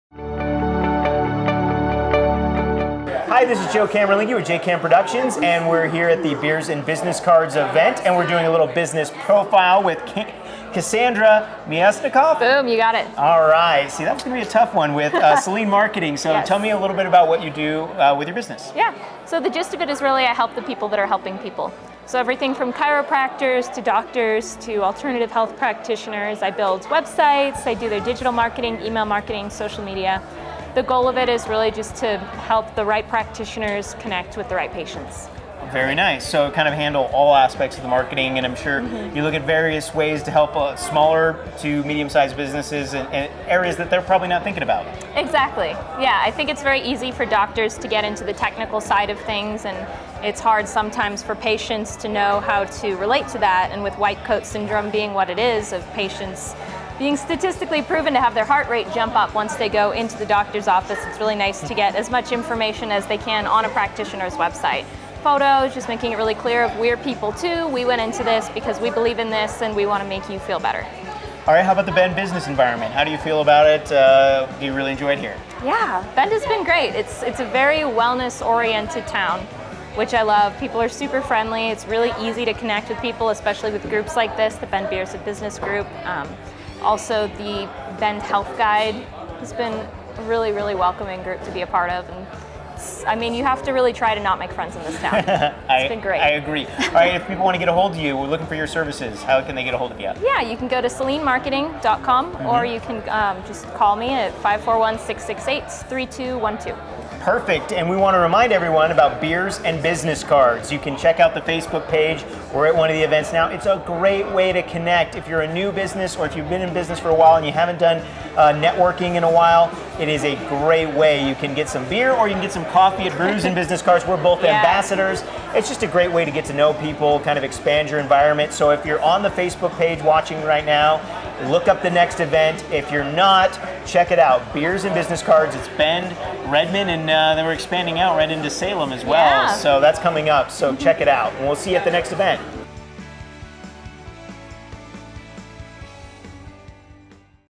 Cassandra Miaksnikov Beers and Business Cards Profile
Joe Camarlinghi interviews Cassandra Miaksnikov of Selene Marketing at a Beers and Business Cards Event.